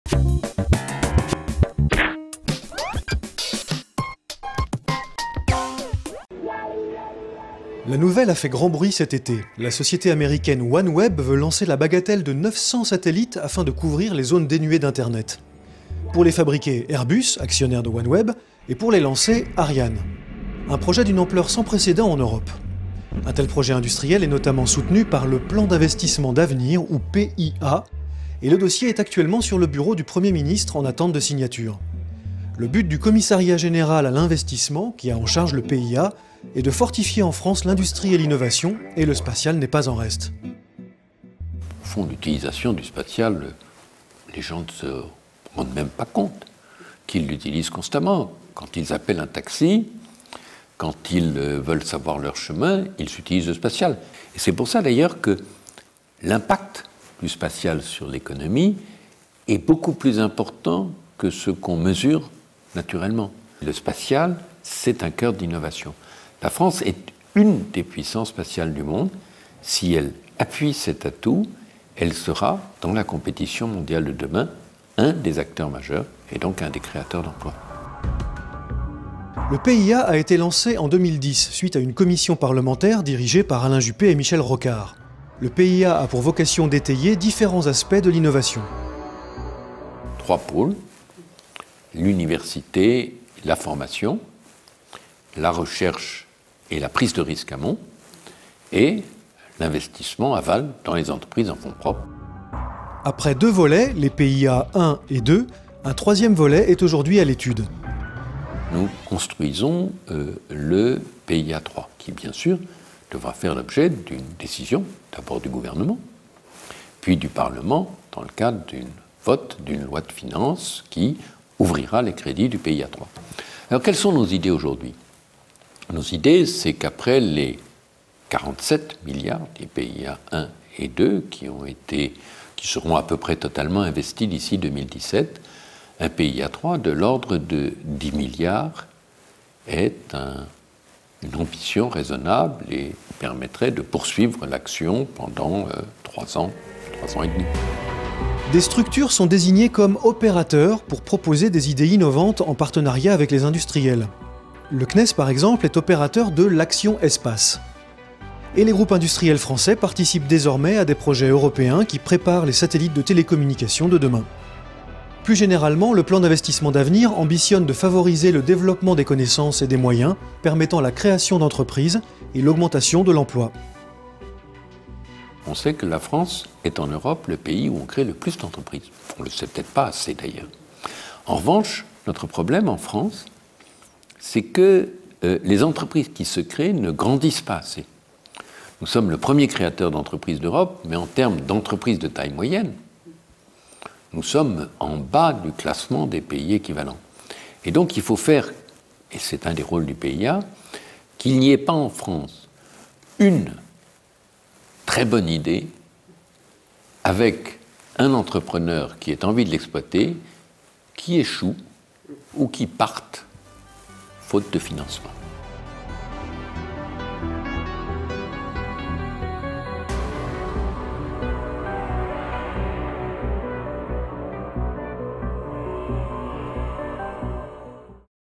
PIA : un soutien pour l’industrie spatiale
La société américaine OneWeb prévoit le lancement de 900 satellites, fabriqués par Airbus et lancés par Ariane, afin de couvrir les zones dénuées d’internet. Ce projet est soutenu par le PIA, Plan d’Investissements d’Avenir. Louis Schweitzer, commissaire général à l’investissement, nous explique pourquoi le PIA doit notamment soutenir l’industrie spatiale. Crédits : CNES.